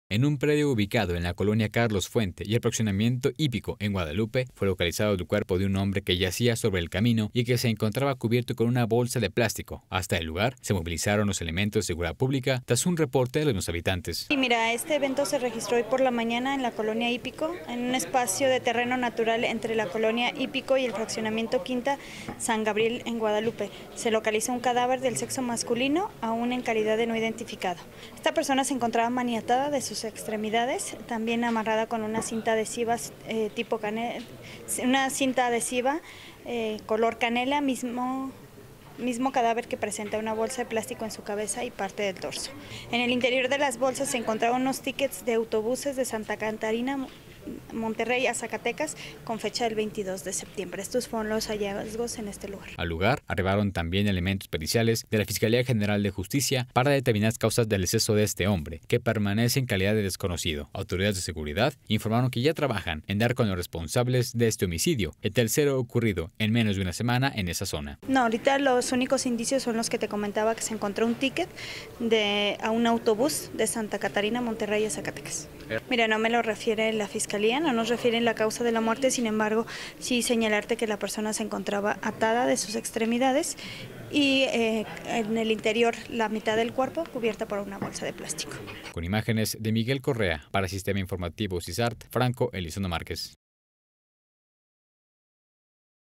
Localizan cuerpo en Guadalupe
En un predio ubicado entre la colonia Carlos Fuente y el fraccionamiento Hípico, en Guadalupe, fue localizado el cuerpo de un hombre que yacía sobre el camino y que se encontraba cubierto con una bolsa de plástico. Hasta el lugar, se movilizaron elementos de Seguridad Pública, tras un reporte de los mismos habitantes.